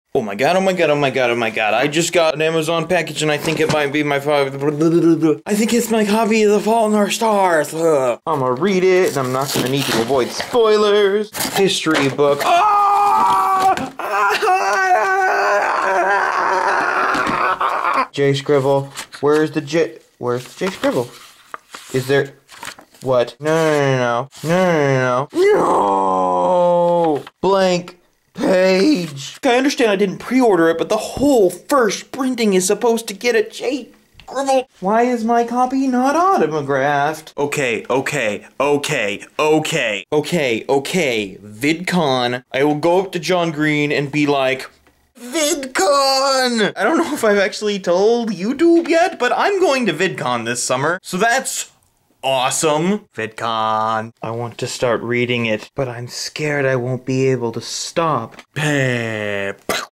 My Unsigned Copy of The Fault In Our Stars
Just to be clear, I'm not upset with John or really even disappointed, but I very much plan to go up to him at VidCon and make him sign book.
Where else am I on the Internets?